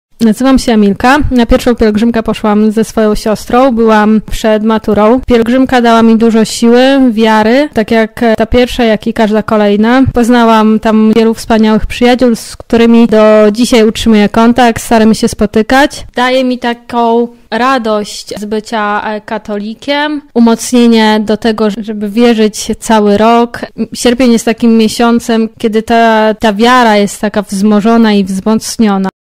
świadectwo Emilii Pielgrzymka Lublin
Seria nagrań, w których ludzie opowiadają o swoich uczuciach, przeżyciach związanych z pielgrzymką

realizacja dźwięku i montaż: Monika Stasieczek